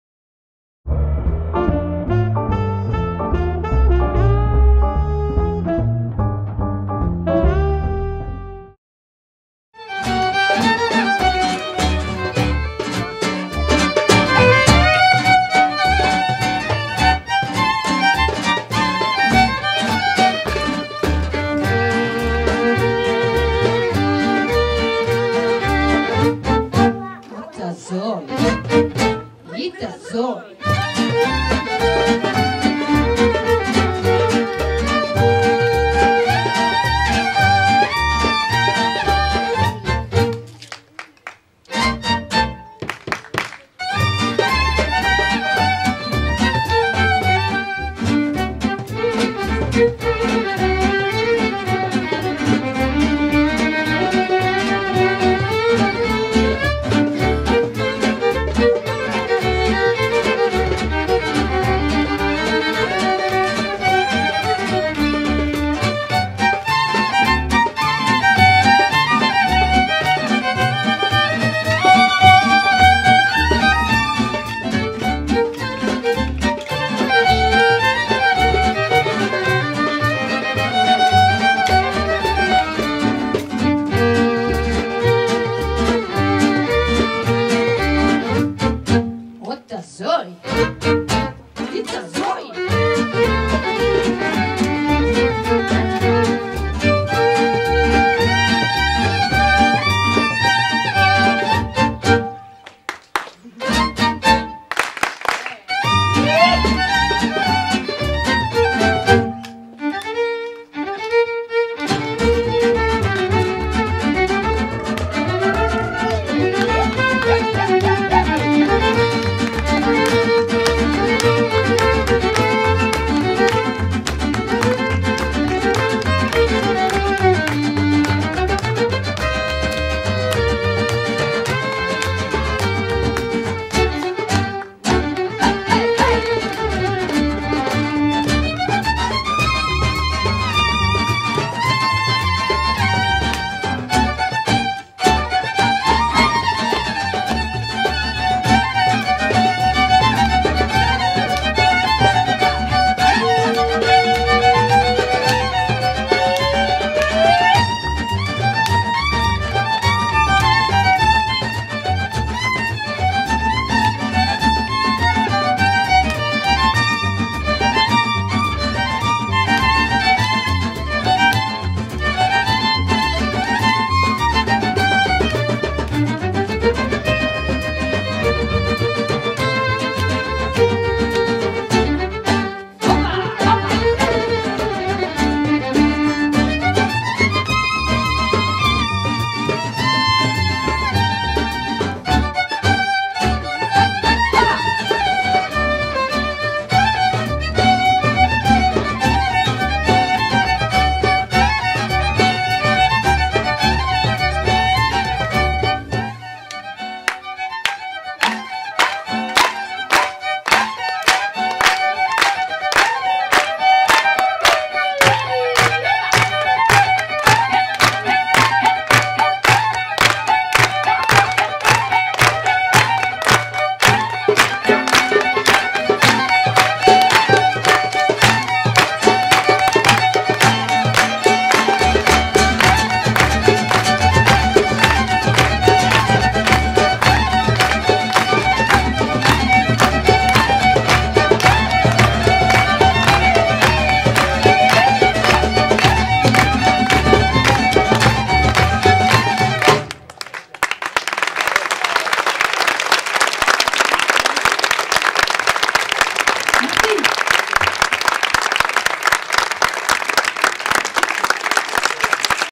BABAYAGA
Musiques tziganes et gitanes, swing manouche et airs de fêtes yiddishs ont peuplé notre après-midi ensoleillé  au rythme imposé par ces quatre filles au tonus débordant.